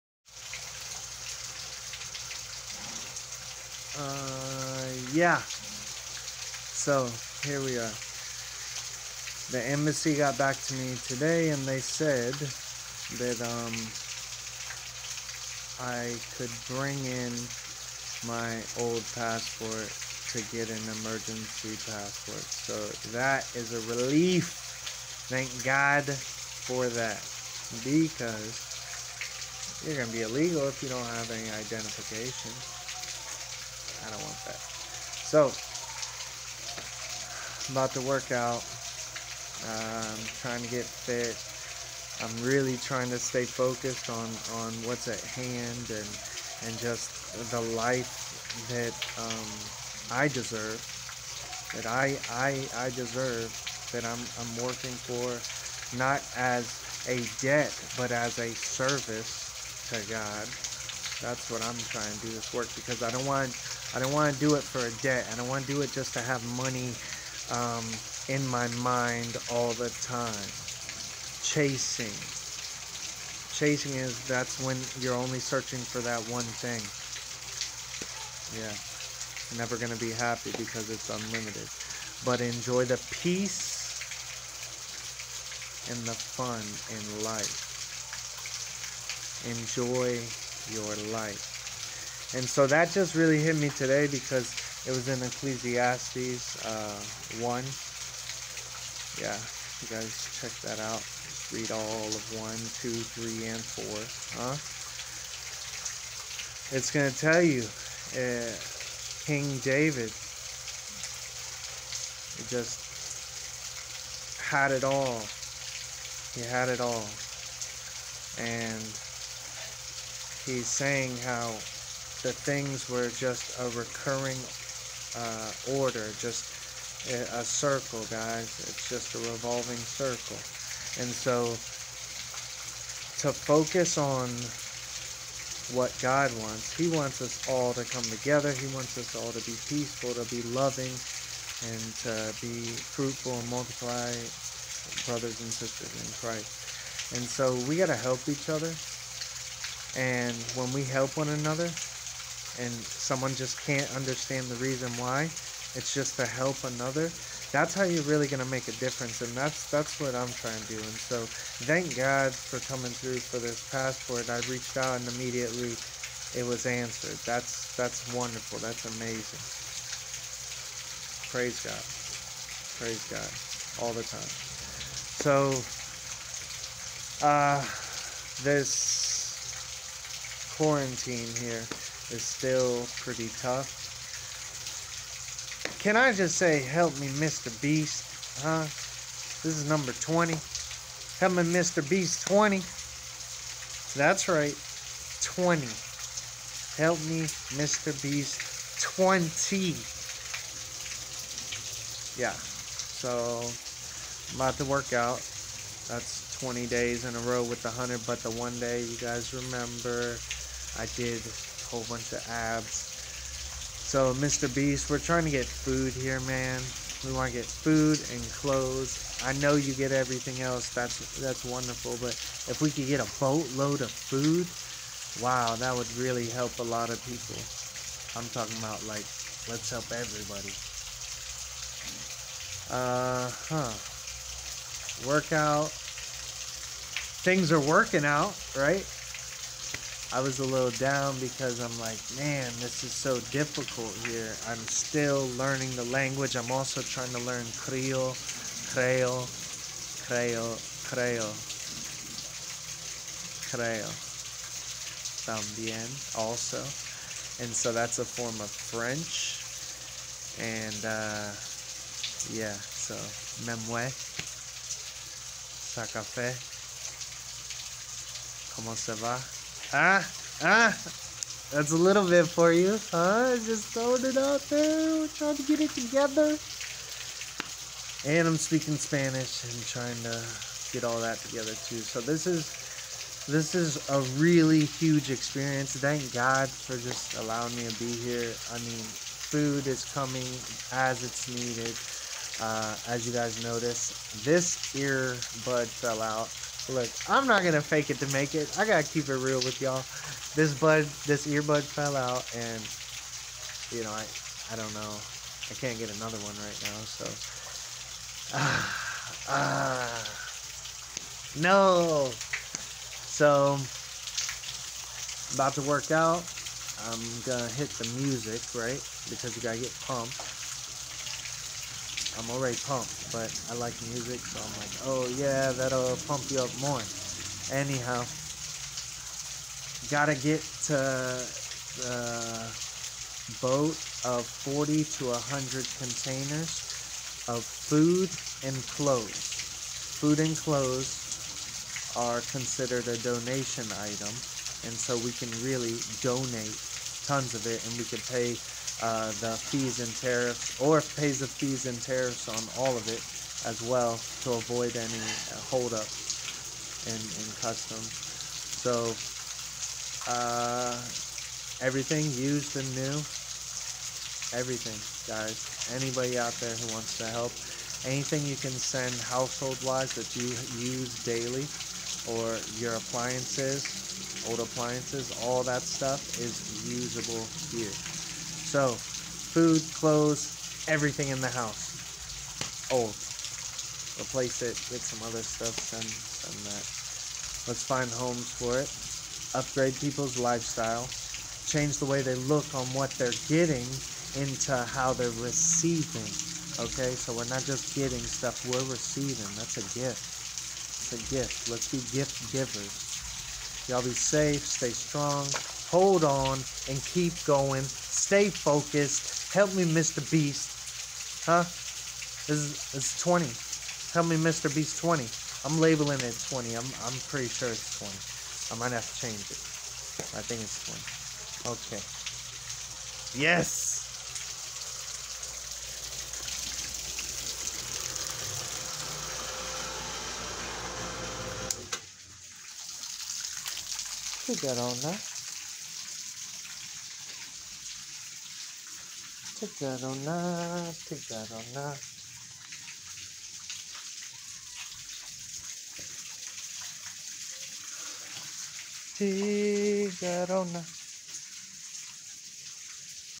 MrBeast HELP me 20 help is on its way
MrBeast HELP me get a boatloqd of food and clothes here to the Dominican republic. Let's go to the factories and have all your merchandise